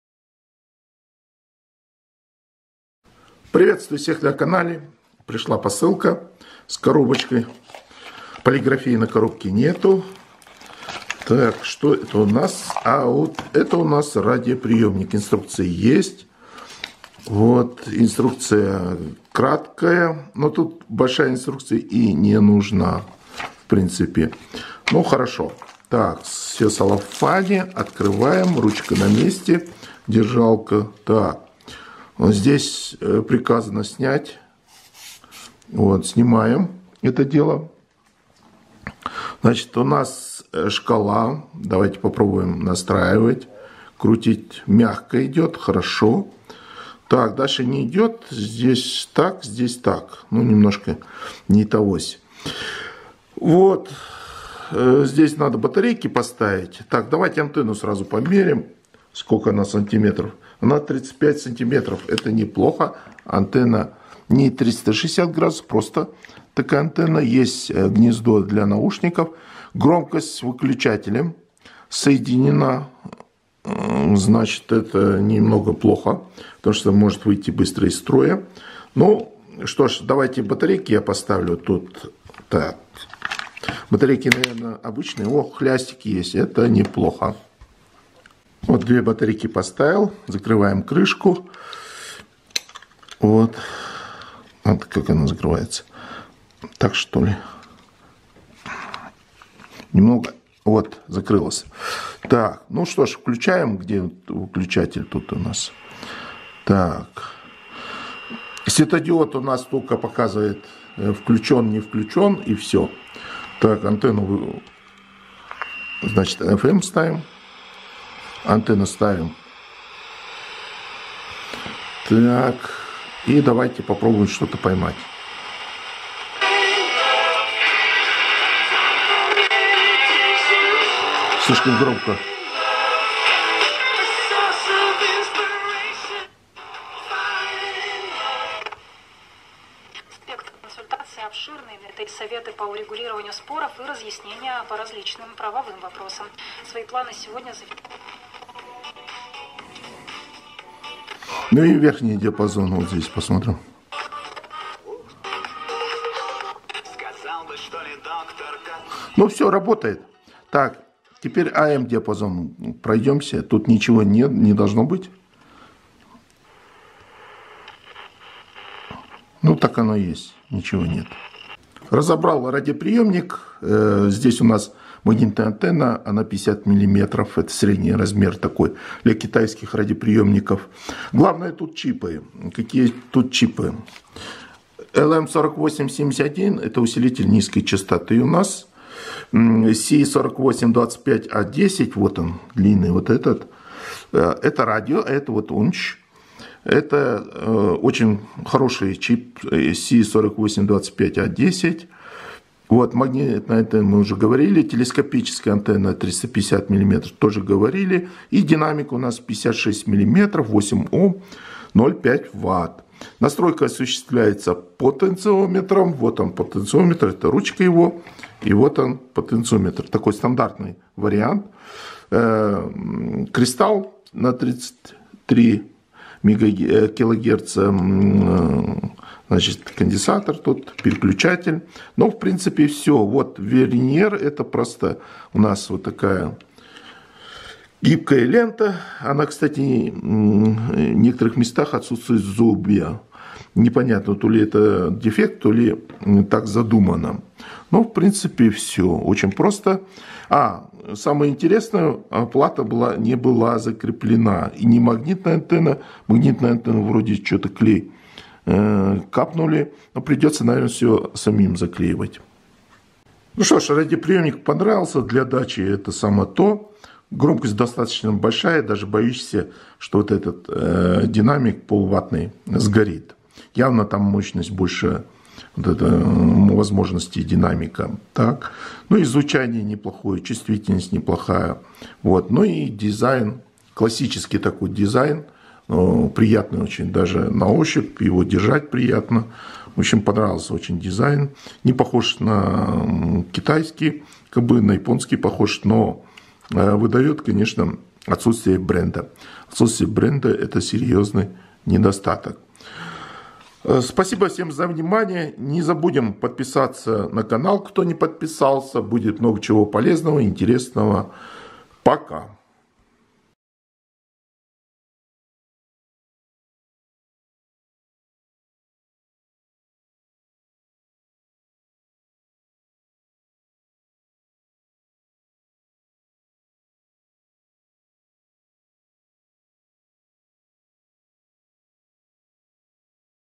Винтажный портативный недорогой FM AM радиоприемник
ПРЕИМУЩЕСТВА - винтажный дизайн, удобный, дешевый,  компактный и легкий, громкий и качественный звук, четкая шкала настройки, удобная настройка крутилкой частоты и громкости, ручка переноски, питание от двух пальчиковых батареек АА, наличие хлястика для извлечения батареек, гнездо под наушники, инструкция на английском, телескопическая антенна 350 мм и магнитная антенна на 50мм, ручка переноски.
НЕДОСТАТКИ - отсутствует бренд производителя, упаковочная коробка без полиграфии, отсутствует светодиодный индикатор точной подстройки на станцию, телескопическая антенна не вращается на 360 градусов, выключатель питания совмещен с регулятором громкости, плата и магнитная антенна не закреплена, моно на наушники.
ЧИПЫ:  SI4825A10 - мультиволновой радиоприемник, LM4871 усилитель низкой частоты